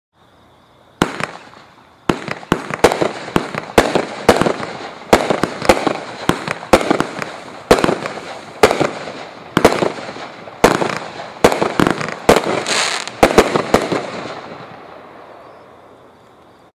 MS437 Milky Way - 16 Shot
Sit back and enjoy the best show in the galaxy!  Palm and brocade crown burst with shining color strobes, followed by a grand finale of red palm with silver crackling stars.